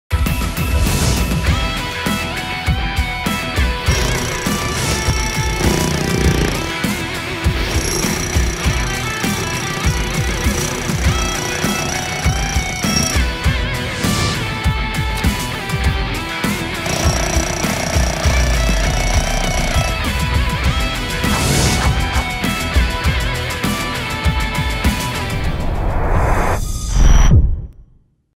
DEMO of Hilti TE 2000-AVR Electric Breaker
Powerful and extremely light TE-S breaker for concrete and demolition work